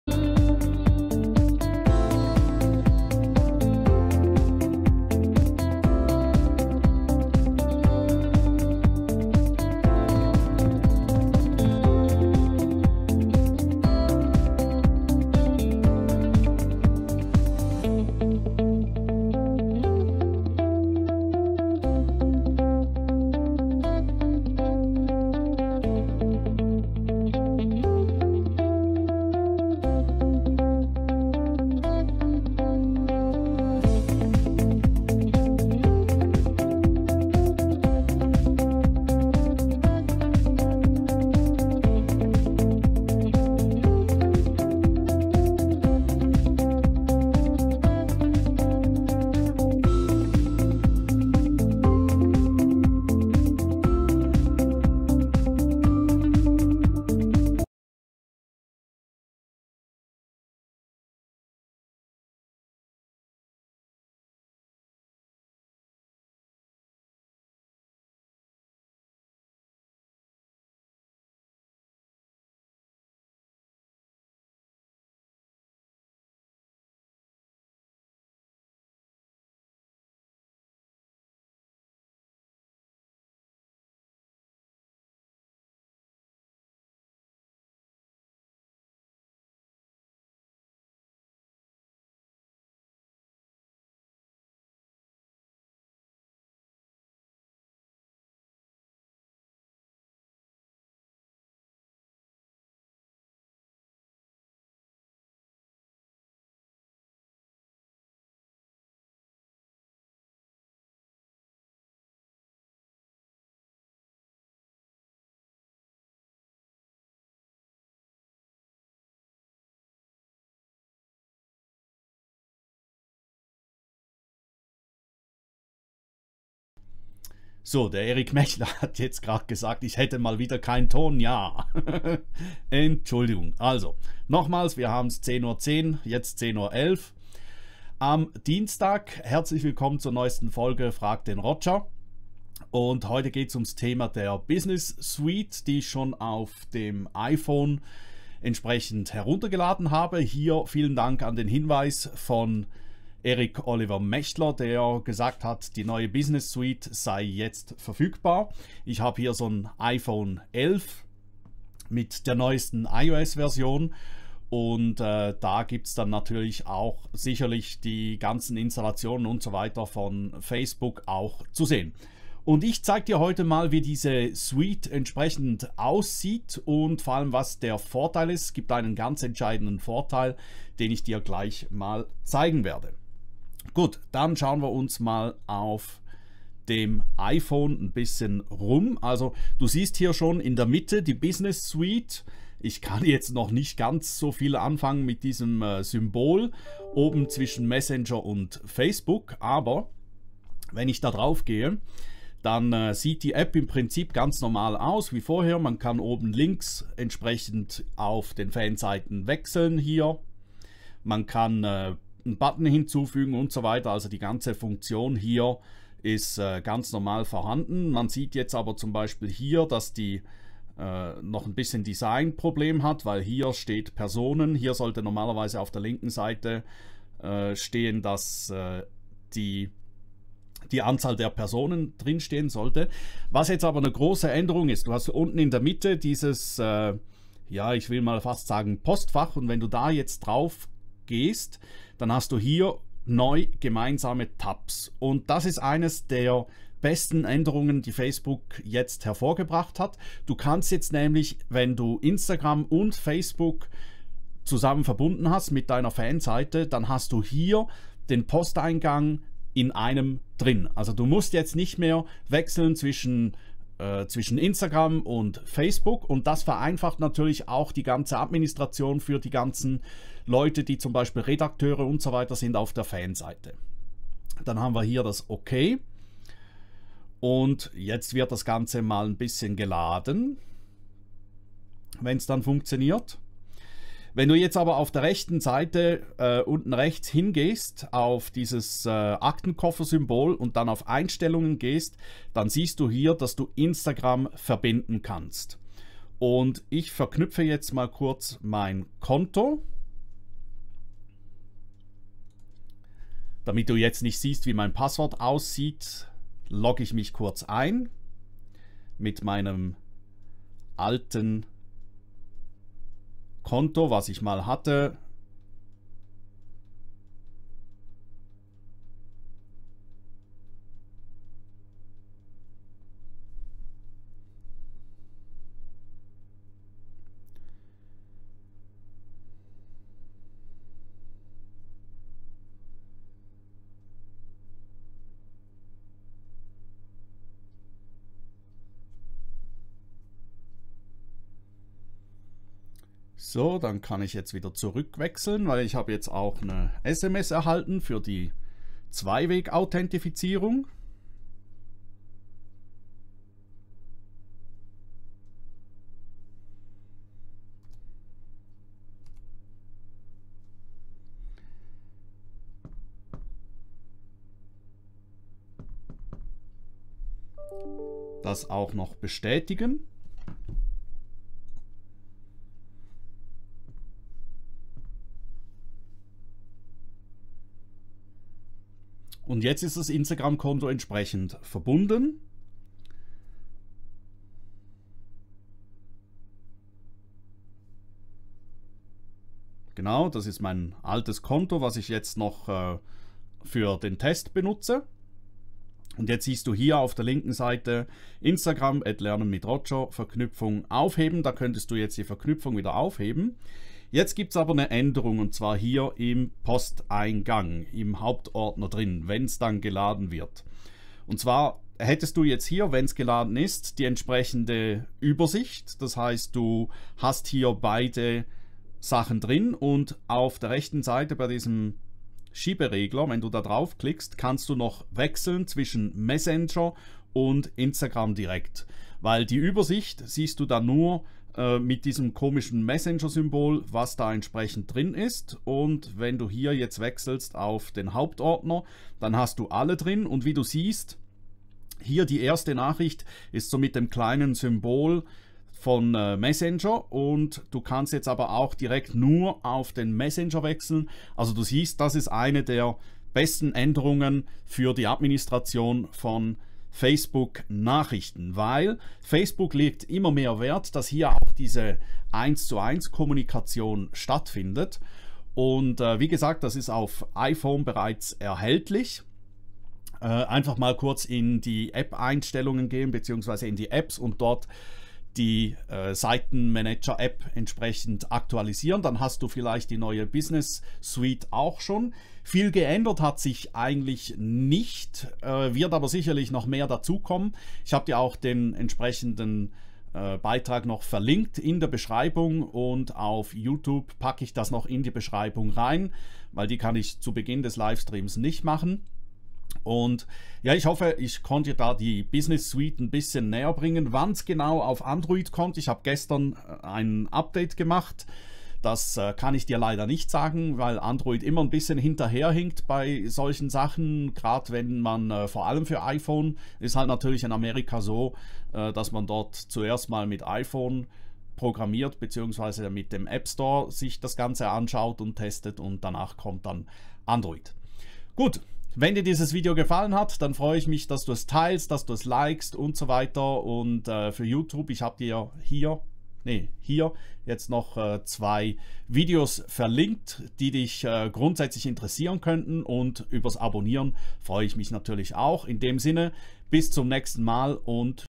Facebook Business Suite - neue iPhone App ersetzt Seitenmanager.
Sorry, Ton war aus. Ab 2:07 gehts dann mit Ton los :-)

Die Facebook Business Suite habe ich letzte Woche in meinen neu erscheinenden Insides (Newsletter) bereits erwähnt.
Heute stelle ich die neue iPhone App von Facebook vor. Hier mein Blogbeitrag dazu

Inhaltsverzeichnis:
0:00 Start des Videos ohne Ton ;-)
2:07 Start mit Ton
3:06 neue iPhone App Facebook Business Suite zeigen
4:44 Beginn Verknüpfung Instagram
5:02 kurze Blende auf mich bei Verknüpfung des Kontos
5:45 weiter mit Verknüpfung
6:43 Unterschied Instagram & Messenger im Hauptordner (Übersicht)
8:48 Abspann